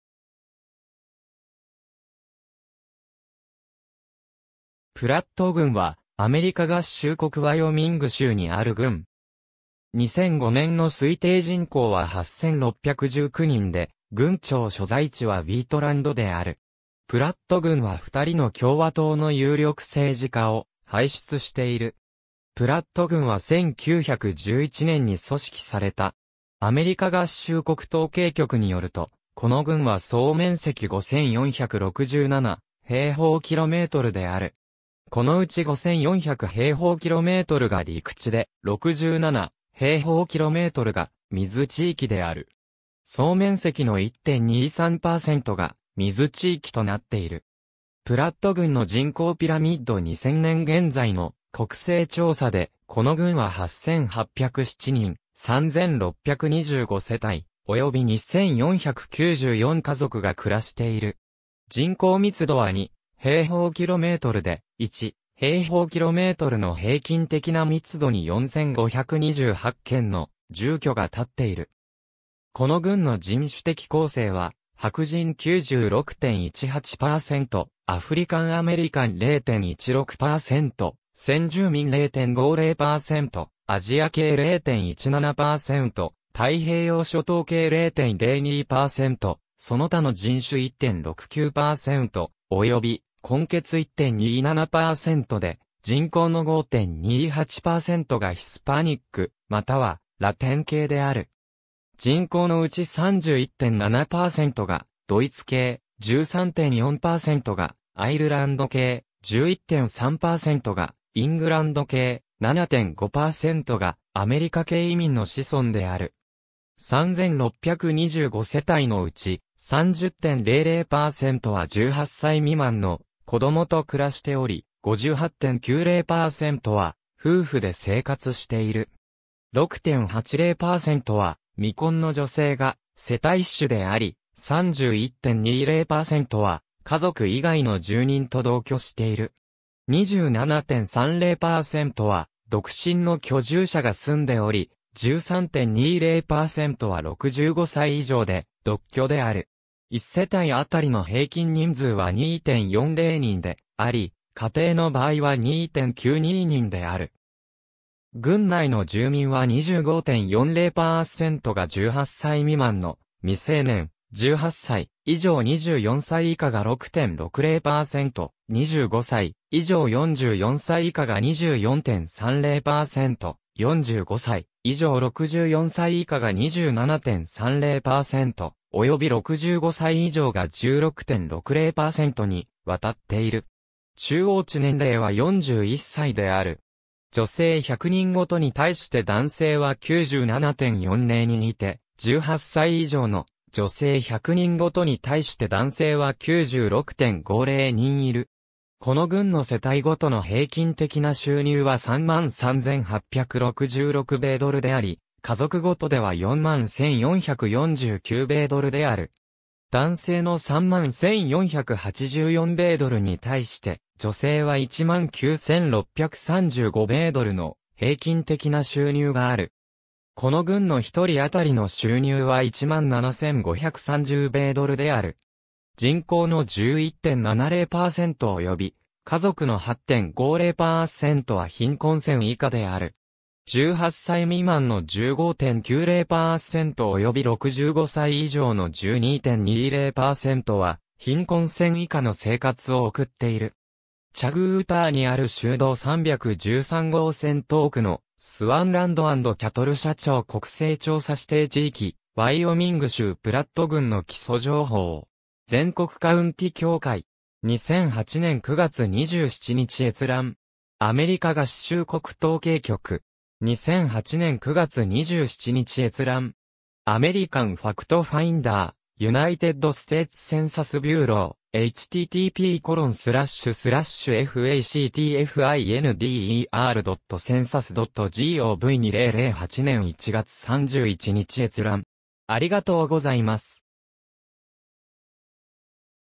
プラット郡 (ワイオミング州)
プラット郡 (英: Platte County) は、アメリカ合衆国ワイオミング州にある郡。
2005年の推定人口は8,619人で、郡庁所在地はウィートランド (Wheatland)である。
プラット郡は2人の共和党の有力政治家を輩出している。
プラット郡は1911年に組織された。
アメリカ合衆国統計局によると、この郡は総面積5,467 km2 (2,111 mi2) である。
このうち5,400 km2 (2,085 mi2) が陸地で、67 km2 (26 mi2) が水地域である。
プラット郡の人口ピラミッド 2000年現在の国勢調査で、この郡は8,807人、3,625世帯、及び2,494家族が暮らしている。
この郡の人種的構成は白人96.18%、アフリカン・アメリカン0.16%、先住民0.50%、アジア系0.17%、太平洋諸島系0.02%、その他の人種1.69%、及び混血1.27%で、人口の5.28%がヒスパニックまたはラテン系である。
人口のうち31.7%がドイツ系、13.4%がアイルランド系、11.3%がイングランド系、7.5%がアメリカ系移民の子孫である。
3,625世帯のうち、30.00%は18歳未満の子供と暮らしており、58.90%は夫婦で生活している。
6.80%は未婚の女性が世帯主であり、31.20%は家族以外の住人と同居している。
27.30%は独身の居住者が住んでおり、13.20%は65歳以上で独居である。
1世帯あたりの平均人数は2.40人であり、家庭の場合は2.92人である。
郡内の住民は25.40%が18歳未満の未成年、18歳以上24歳以下が6.60%、25歳以上44歳以下が24.30%、45歳以上64歳以下が27.30%、及び65歳以上が16.60%にわたっている。
この郡の世帯ごとの平均的な収入は33,866米ドルであり、家族ごとでは41,449米ドルである。
18歳未満の15.90%及び65歳以上の12.20%は貧困線以下の生活を送っている。
チャグウォーターにある州道313号線と奥のスワンランド&キャトル社 町 国勢調査指定地域 ^ “ワイオミング州プラット郡の基礎情報”. 全国カウンティ協会. 2008年9月27日閲覧。
^ “アメリカ合衆国統計局”. 2008年9月27日閲覧。